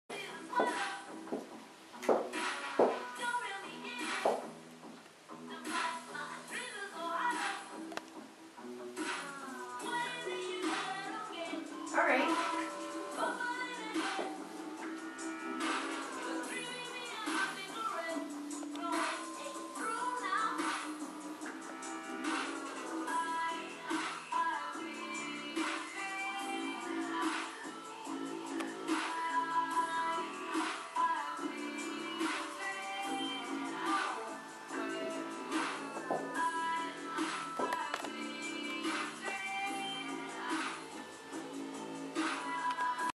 Behind the Scenes: Hair and makeup photo Shoot 2011
Quick clip of model Stefanie Ball during a photo shoot.  Makeup by Jeff Enriquez, hair by Giselle Chreky.